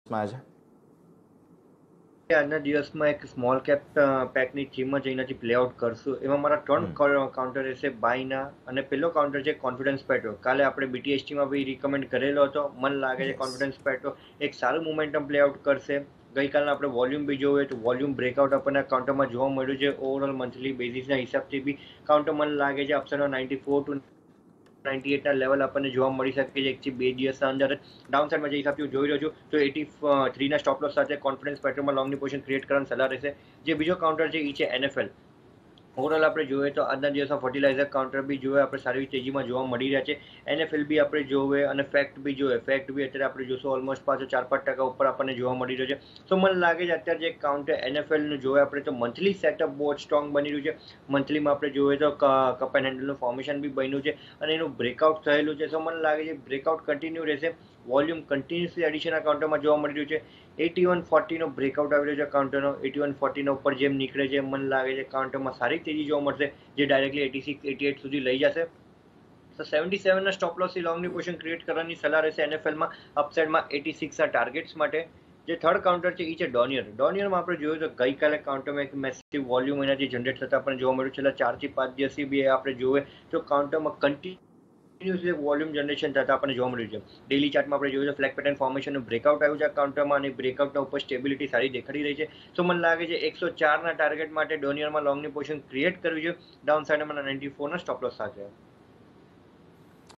Buy Confidence Petroleum, NFL & Donear Industries| Trading call by Amit Bhuptani
CNBCBajar | Market | આજે ક્યાં સ્ટોક કરાવશે કમાણી? જાણો નિર્મલબંગ સિક્યોરિટીઝના અમિત ભૂપતાની પાસેથી